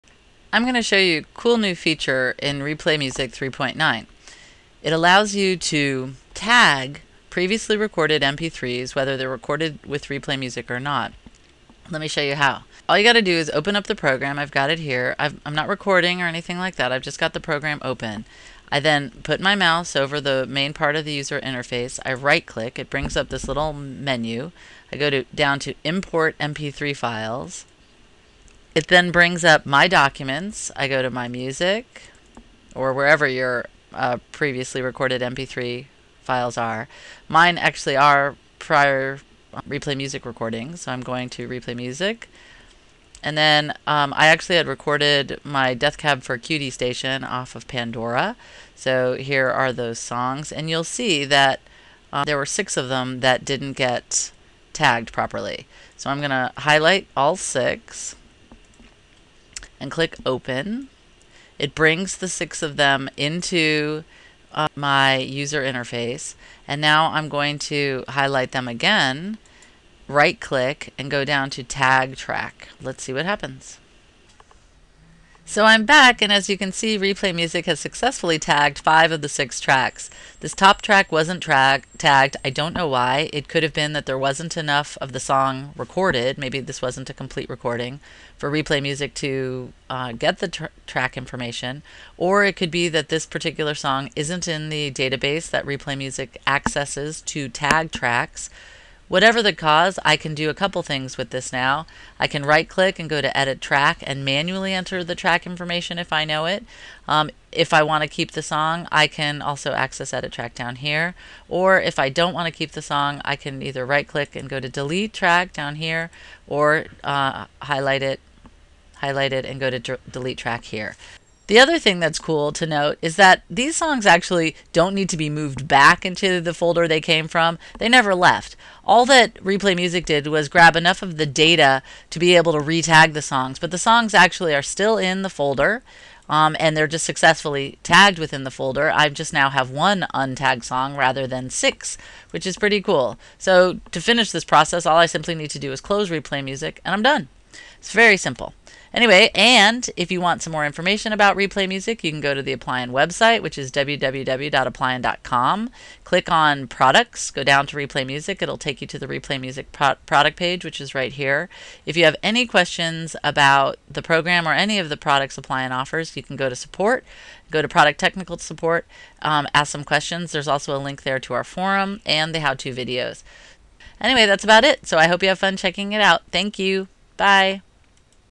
Audio Recorder for MP3s and Music: Tag/Rename/Edit MP3s Previously Recorded Mp3s using Replay Music
Audio Recorder and Tagger.  Replay Music 3.9 allows you to import previously recorded MP3s and tag them using the new import MP3 and tag feature.

Watch this video to learn how to import MP3s recorded from Pandora and tag/edit/rename them using Replay Music.